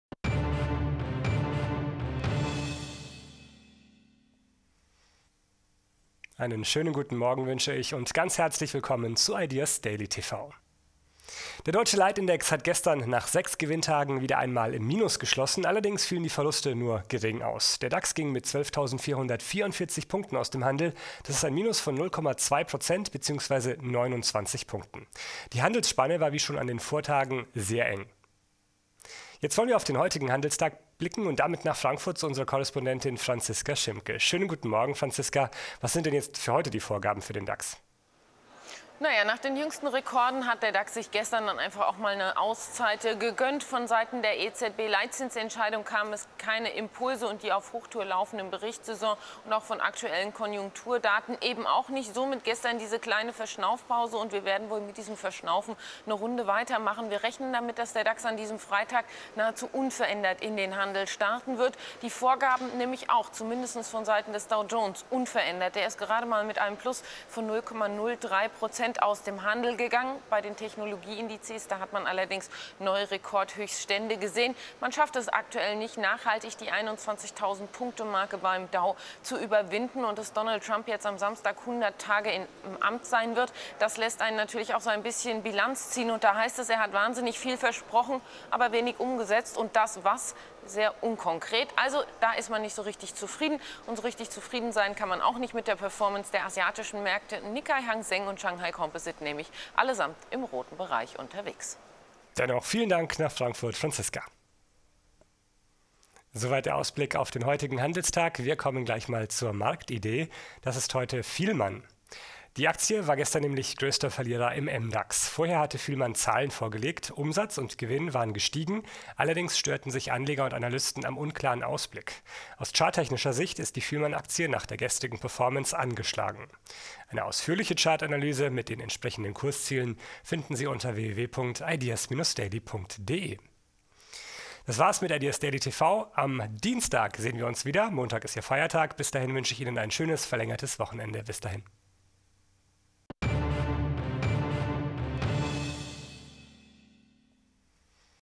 Ideas Daily TV: DAX - Pattsituation / Marktidee - Fielmann
Der deutsche Leitindex hat gestern nach sechs Gewinntagen in Folge wieder einmal im Minus geschlossen. Allerdings fielen die Verluste nur gering aus. Der DAX ging mit 12.444 Punkten aus dem Handel. Das ist ein Minus von 0,2 Prozent bzw. 29 Punkten. Die Handelsspanne war wie schon an den Vortagen sehr eng.